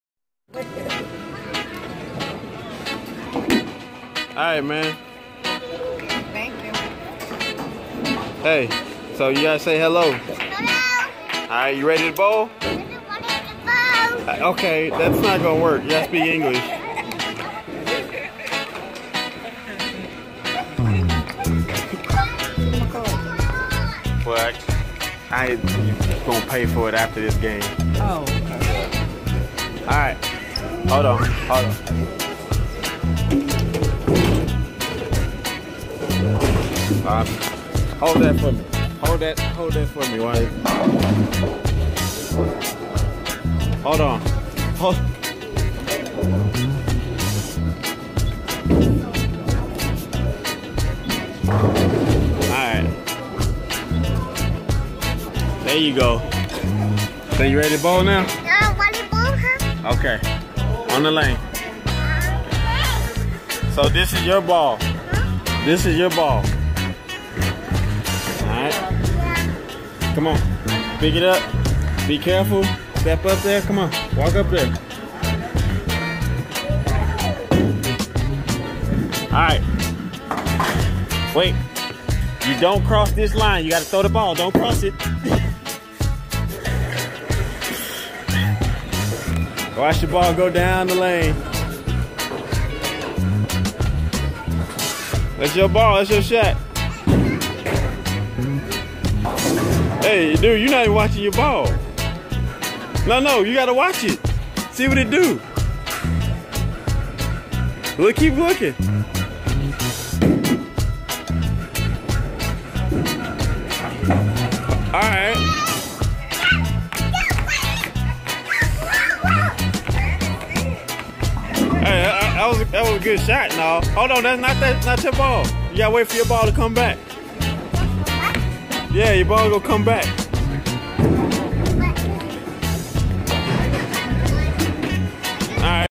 Bowling with my cousin.
Spending with my cousin is always special, you never know what to expect. Lol Please forgive me being in the  way briefly.  I hope this video makes you smile.